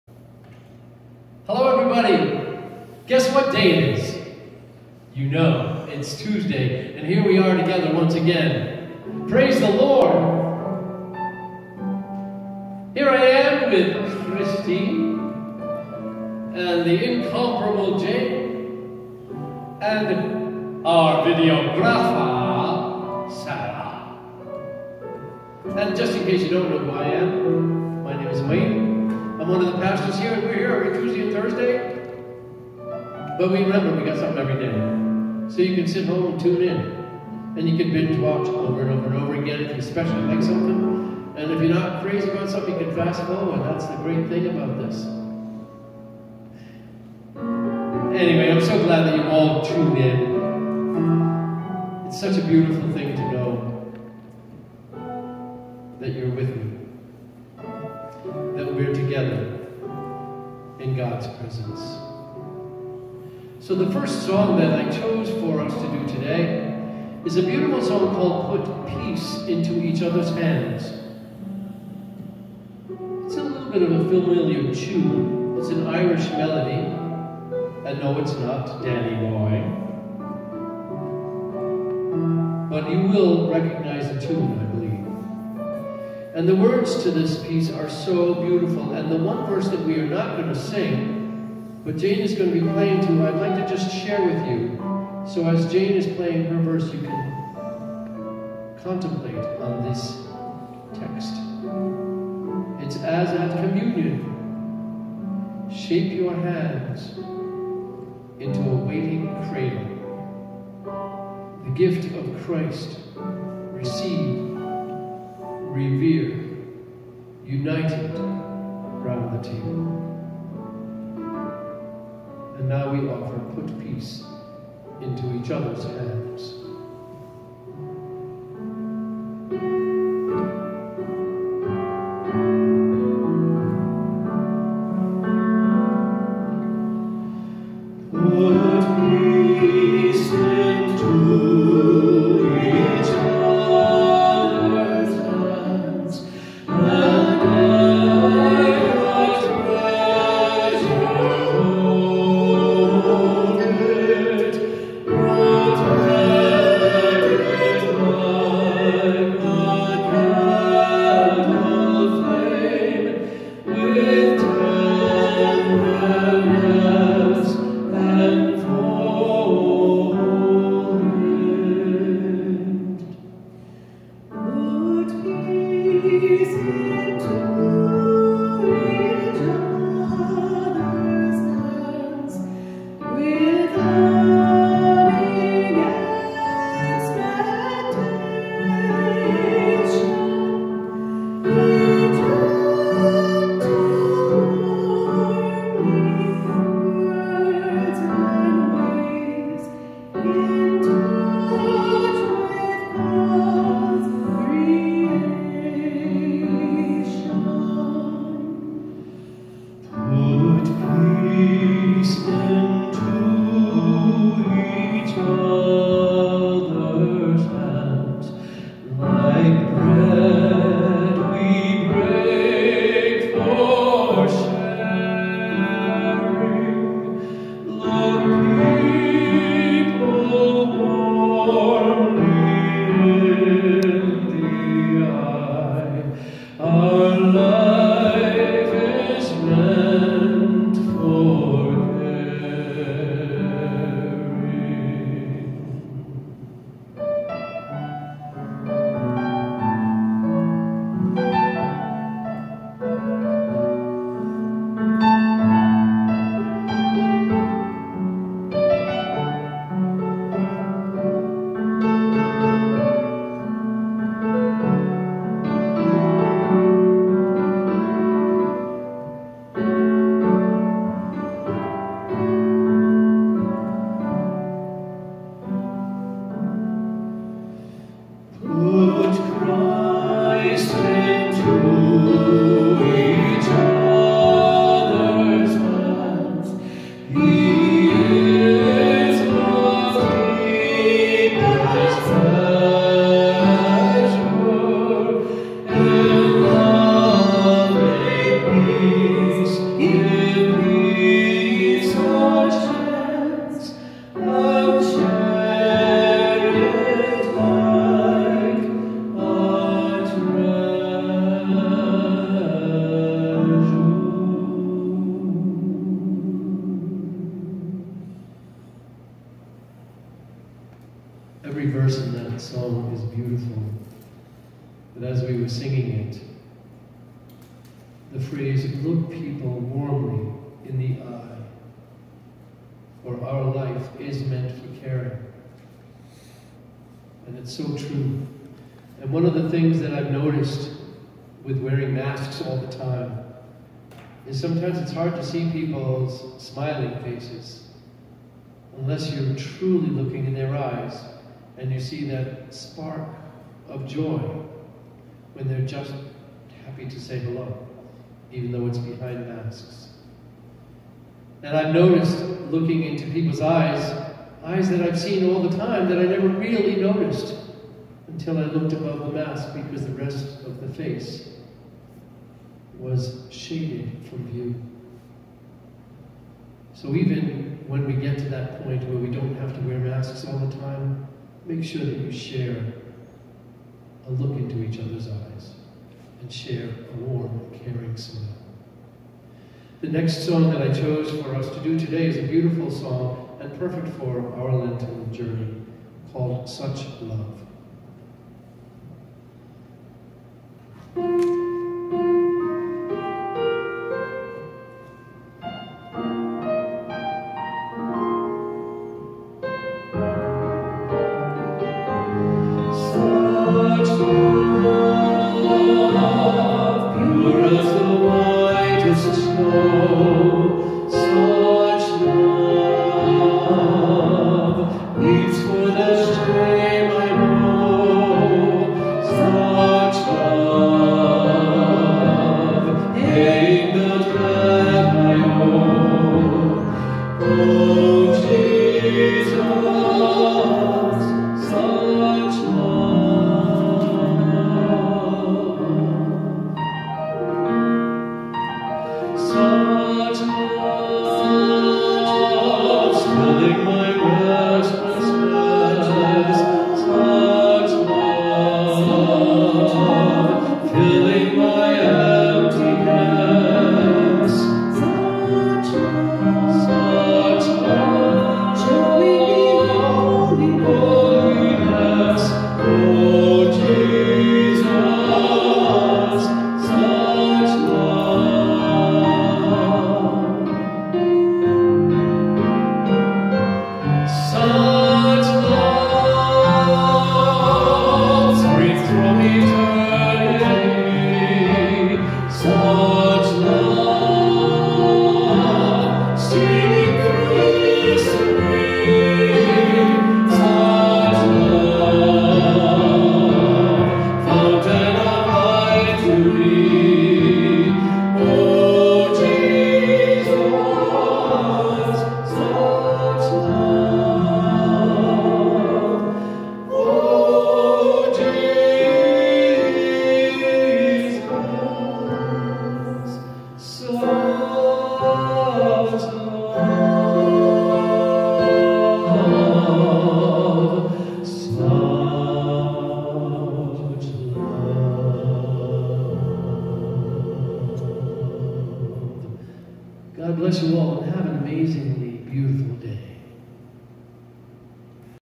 Words & Music - Tuesday 3- 2- 2021
Welcome to Words & Music with Pastor Wayne and Kristine Gervais with Jane Bedia on keyboard. Come, join in singing praises with us!
Sara Morrissette-videographer.